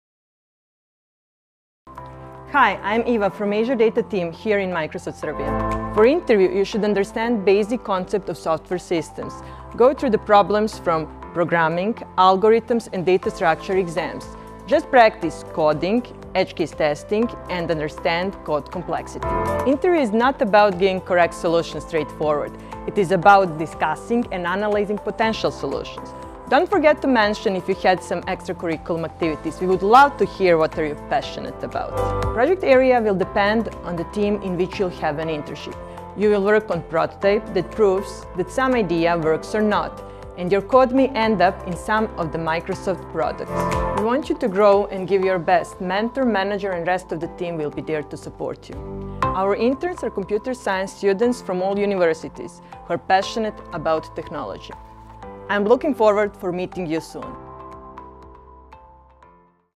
MDCS Internship - What to expect?
Iva from the Azure Data Team, intern manager of our interns, explained how to prepare for an interview and what to expect when applying for student opportunities here at MDCS. If you are passionate about technology, join us and visit aka.ms/student-opportunities